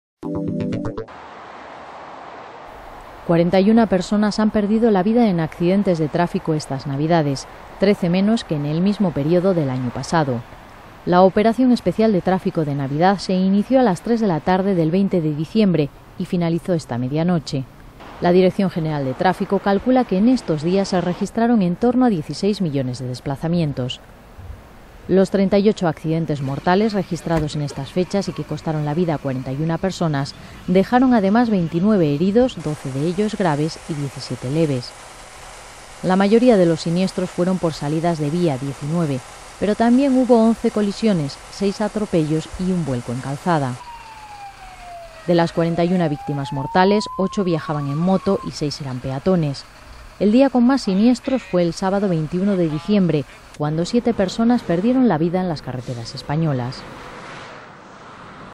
Mueren 41 personas en las carreteras en Navidad, 13 menos que el año anterior
Madrid, 7 ene (EFE).- Cuarenta y una personas han muerto en los 38 accidentes mortales ocurridos en las carreteras españolas desde las tres de la tarde del 20 de diciembre, cuando arrancó la operación especial de tráfico de Navidad, hasta su finalización a las doce de la noche de ayer.